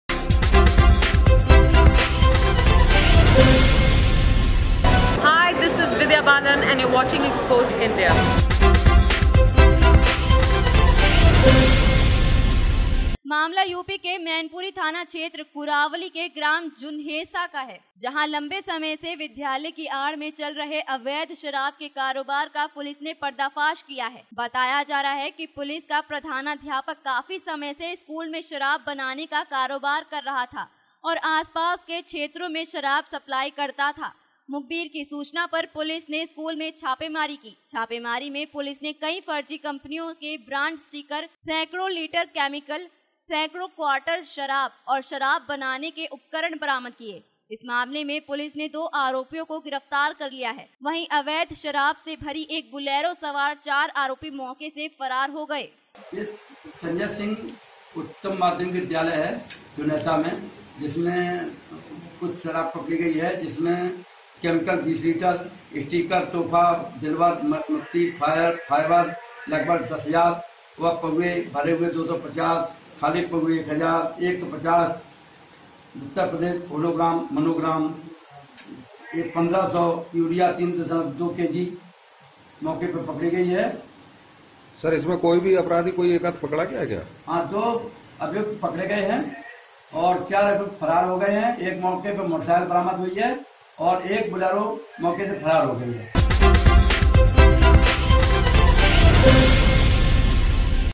स्कूल में चल रहा शराब बनाने का कारोबार
मामला यूपी के मैनपुरी थाना क्षेत्र कुरावली के ग्राम जुन्हैसा का है. जहां लंबे समय से विद्यालय की आड़ में चल रहे अवैध शराब के कारोबार का पुलिस ने पर्दाफाश किया है….बताया जा रहा है कि स्कूल का प्रधानाध्यापक काफी समय से स्कूल में शराब बनाने का कारोबार कर रहा था. और आस-पास के क्षेत्रों में शराब सप्लाई करता था. मुखबीर कि सूचना पर पुलिस ने स्कूल में छापेमारी की. छापेमारी में पुलिस ने कई फर्जी कम्पनियों के ब्रांड स्टीकर, सैकड़ों लीटर केमिकल, सेकड़ों क्वाटर शराब, और शराब बनाने के उपकरण बरामद किये. इस मामले में पुलिस ने दो आरोपियों को गिरफतार कर लिया है. वहीं अवैध शराब से भरी एक बुलेरो सवार चार आरोपी मौके से फरार हो गए.